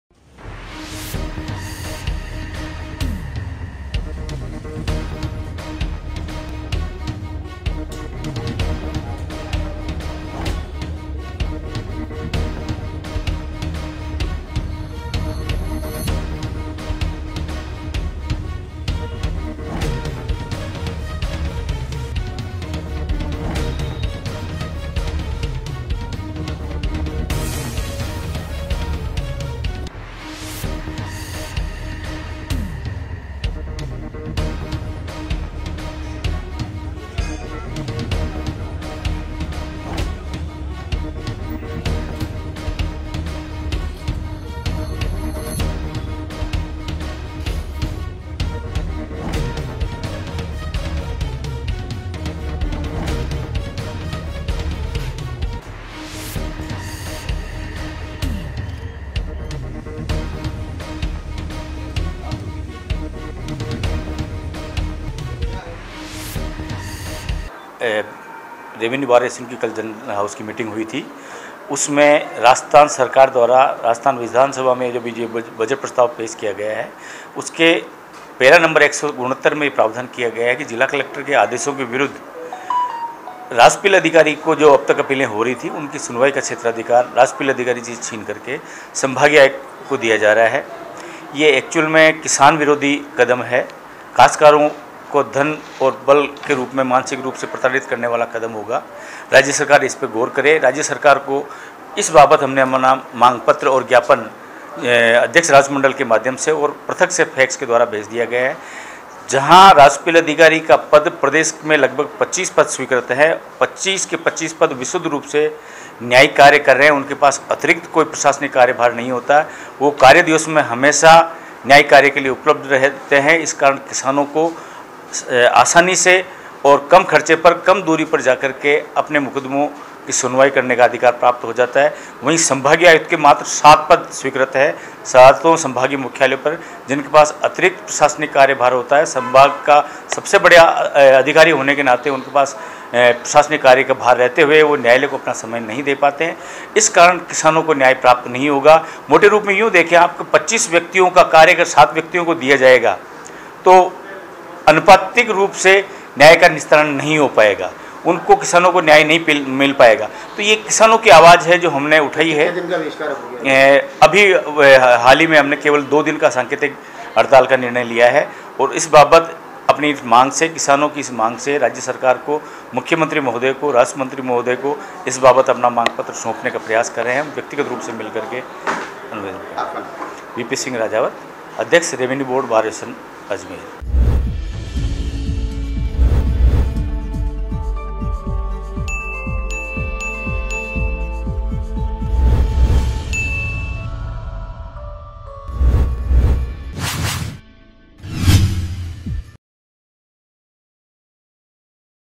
AJMER NEWS | राजस्व मंडल में वकीलों ने किया न्यायिक कार्य का बहिष्कार | MTTV INDIA
राजस्व मंडल में वकीलों ने किया न्यायिक कार्य का बहिष्कार
आज से दो दिन रहेगा कार्य बहिष्कार
राजस्व न्यायालय के अपीलीय क्षेत्राधिकार में परिवर्तन संबंधी
बजट घोषणा के विरोध में किया कार्य बहिष्कार
सरकार से प्रस्ताव वापस लेने की मांग